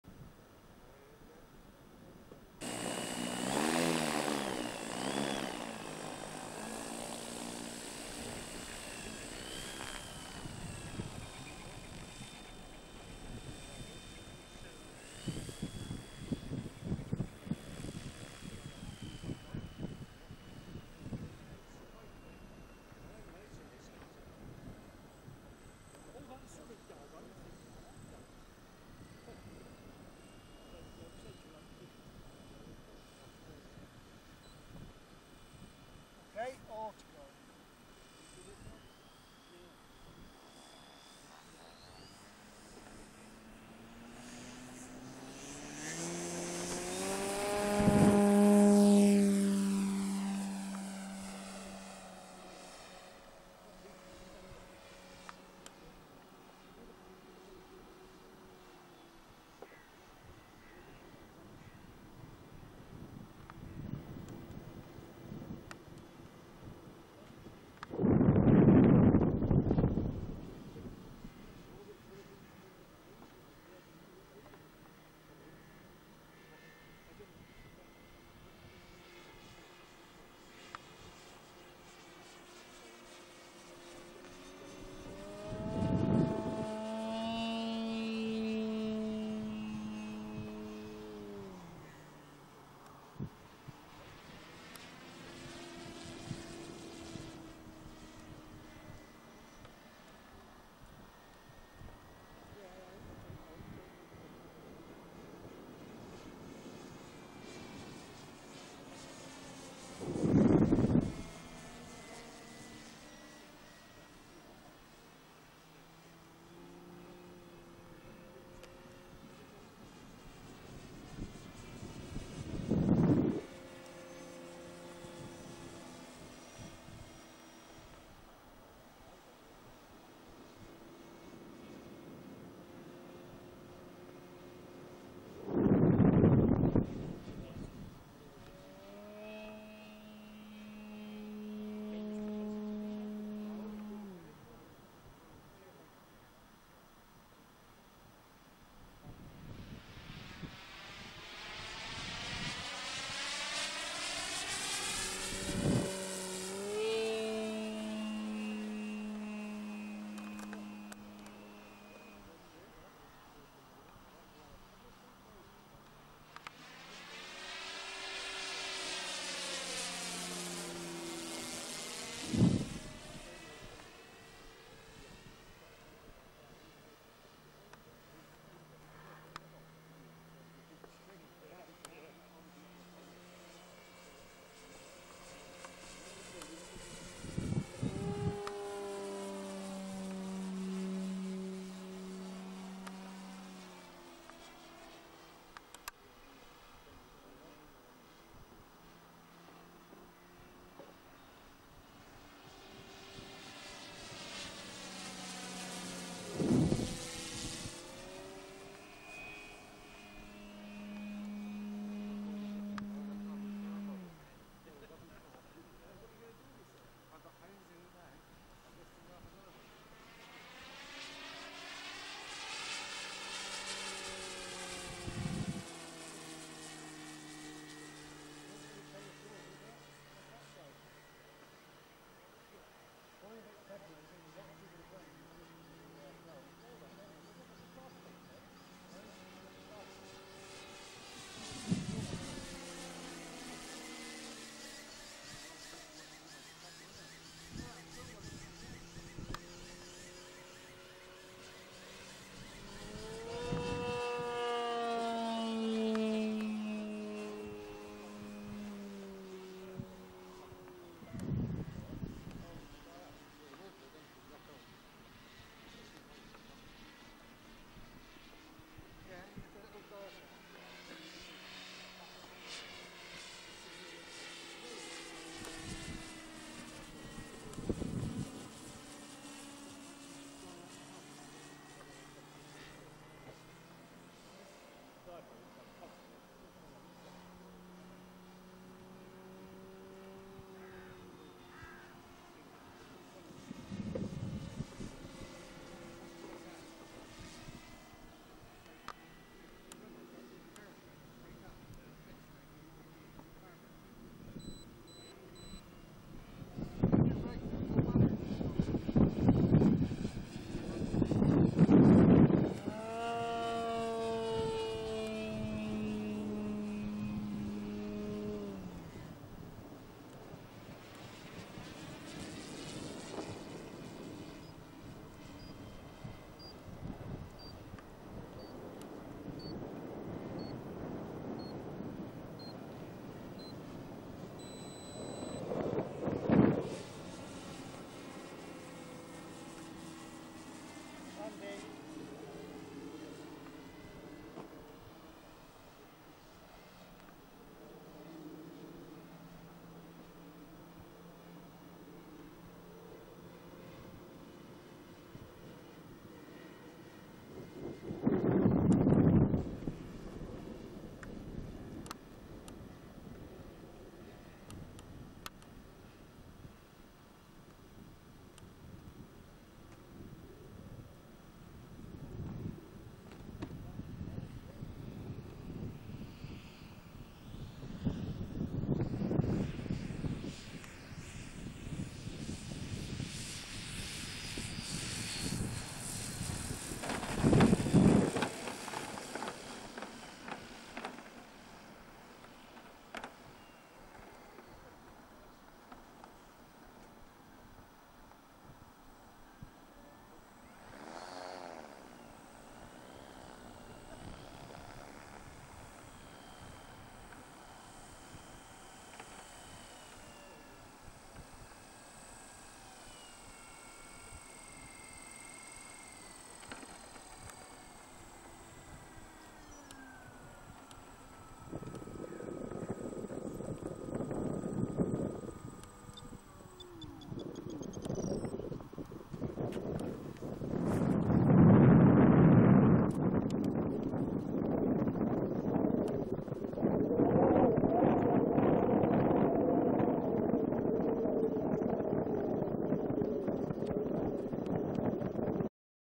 Phoenix Models VOLCANO 60cc (Tucano)
Supplied by Boomerang Jets
Scale 1:4.25
Wingspan 2567mm or 101"
All Up Weight: 14Kg (31.8lbs)
Electric Setup:
Plettenberg Predator 25/8 Motor
Jeti Spin 200 ESC
Emcotec 120/240A SPS
12S Lipo (2 x Turnigy 6S 5000mA A Spec G2)
24x12 JXL Prop
Flown by Gordon Hill
Video by Steve Fish